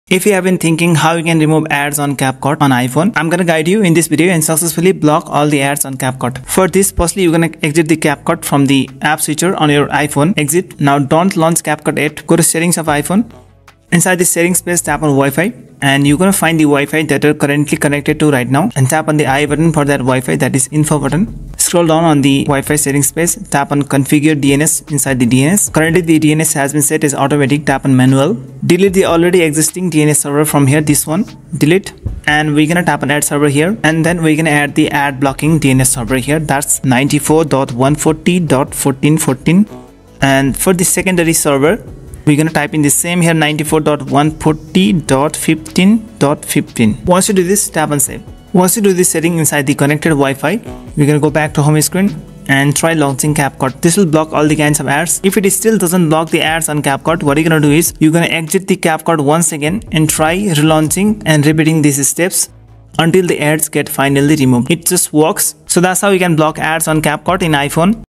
How to Remove Ads on Capcut iPhone | Block Capcut Ads on iPhone
Video showing how you can remove ads on Capcut for iPhone. Block ads on Capcut for iPhone using this simple iPhone ad blocking DNS server.